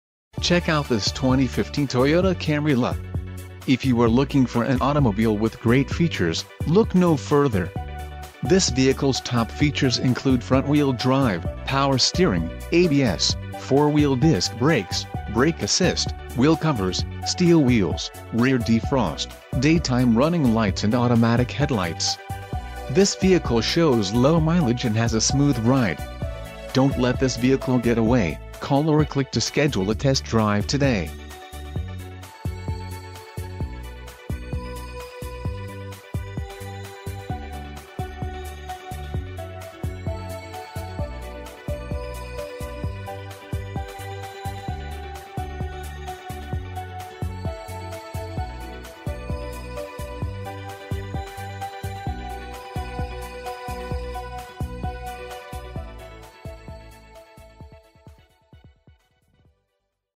2015 Toyota Camry LE - Stadium Toyota - Tampa, FL 33614
Introducing our bold new 2015 Toyota Camry LE in Predawn Gray Mica. A 2.5 Liter 4 Cylinder engine pairs with a smooth-shifting 6 Speed automatic transmission and generates 178hp on demand. This efficiency rewards you with nearly 35mpg on the open road. Solid engineering and high-grade workmanship continue to earn this car accolades every year! The exterior of the LE is accented by a dynamic shape, automatic headlamps and distinctive tail lights. The ergonomically designed interior features a 6.1-in. touch-screen Entune Audio display with Bluetooth phone and a six-speaker sound system with steering wheel mounted controls. You'll also appreciate remote key-less entry, cruise control, a rearview camera, a tilt-and-telescoping steering wheel and split-folding rear seats. Confident handling, straightforward technology and lots of comfortable passenger space make this car an absolute delight to own.